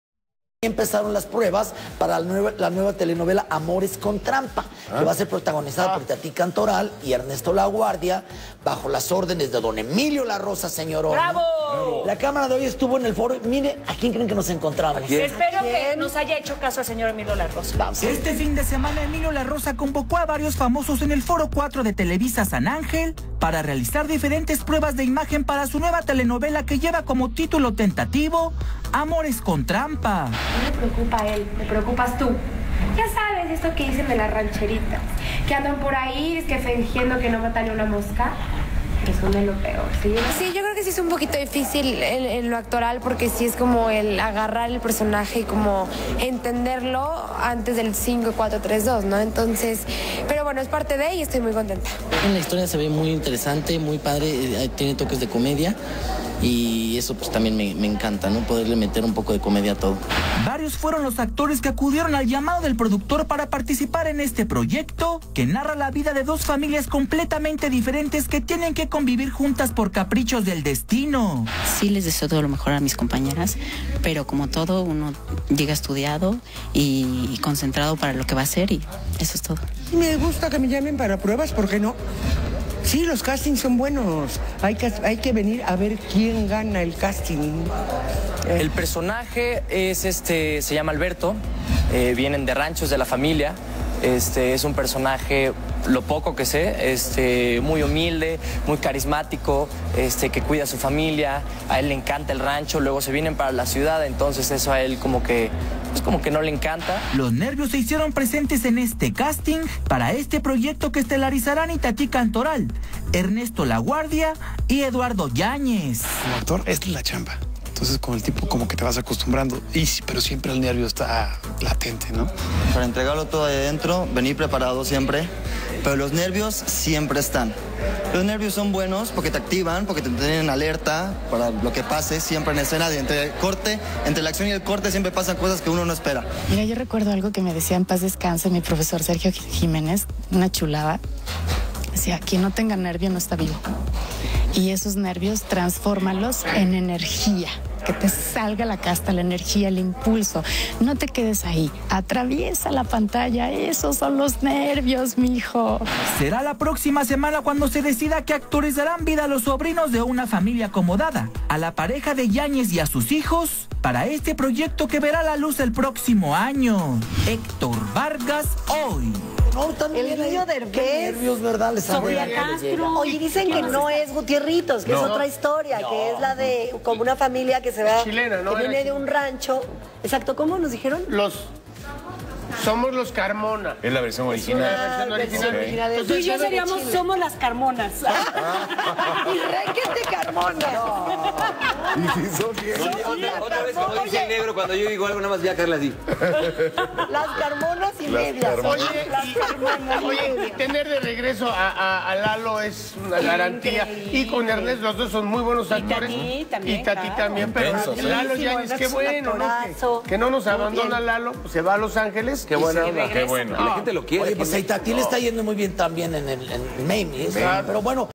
Se realizaron pruebas para la nueva producion de Emilio Larrosa
Casting de la nueva telenovela de Emilio Larrosa donde participara Eduardo Yañez